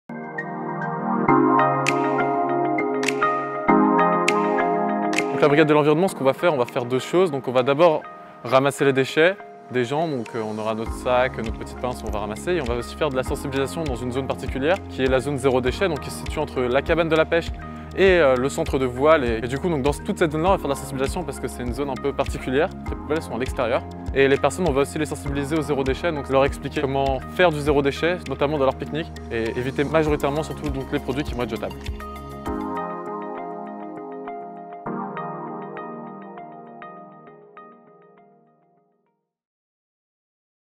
[ Déchets ] Zone zéro déchet à l'Ile-de-Loisirs de SQY
Engagée dans une démarche environnementale forte, l'île de loisirs de Saint-Quentin-en-Yvelines est la première en Île-de-France à avoir mis en place une zone zéro déchet, depuis la mi-mai 2021.
Situé entre le centre de voile et la maison de la pêche, cette zone sans poubelle ni mobilier a pour objectif de sensibiliser les usagers pour en faciliter la gestion des déchets et lui rendre son aspect naturel.